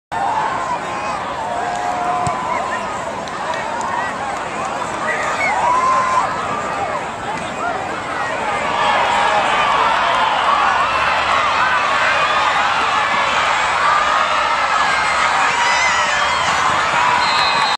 Demarco Thomas, Princeton
Demarco Thomas runs this kick return 73 yards and is pushed out at the Cardinals nine yard line.  The Vikings failed to capitalize on the great field position. Recorded on September 14, 2012 by Michael Collins of TriStateFootball.com using a Flip Video camcorder.